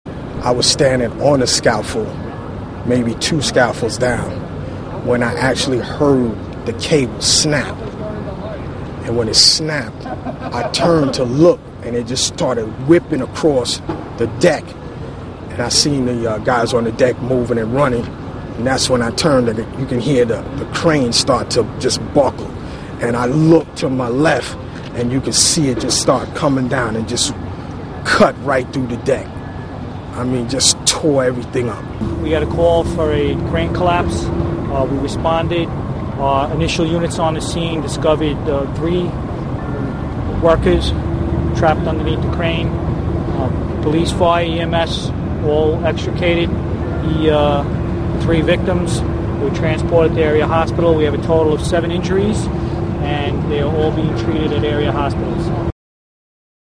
Worker at Crane Collapse 'Heard the Cable Snap'
With the popping of cables and the snapping of metal, a 200-foot crane collapsed onto a building under construction near the East River waterfront Wednesday, injuring seven people. (Jan. 9)